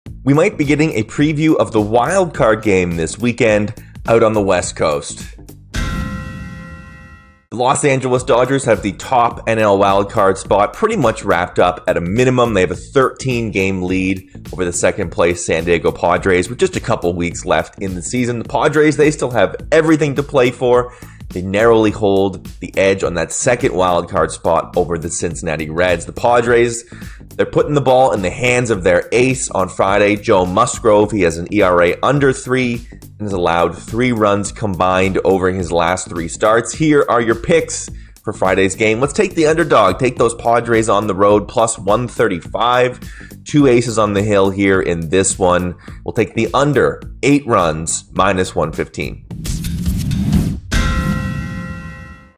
Padres turn to their ace in possible Wild Card preview against Dodgers
The San Diego Padres are looking to solidify their playoff hopes in a possible Wild Card preview against the Los Angeles Dodgers.